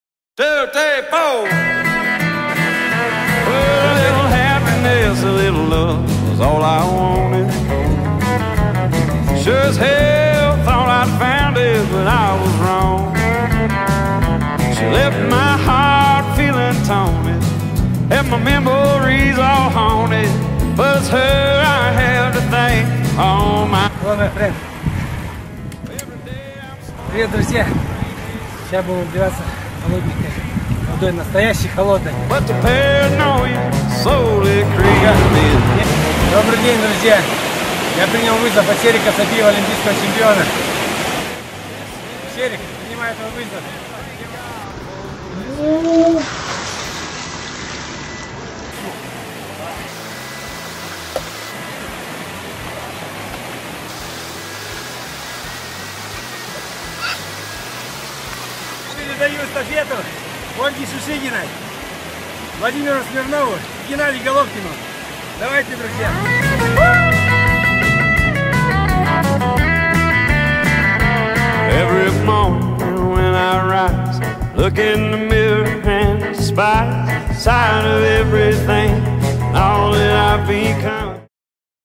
Айс бакэт приколы с Aлександром Bинокуровым
Чемпион Лондонской Олимпиады 2012,
Генеральный менеджер Astana Pro Team принял участие в акции ice bucket challenge -

Эстафету он принял от еще одного Чемпиона Олимпиады, боксера Серика Сапиева.

Кто дальше?

смотрим видео!!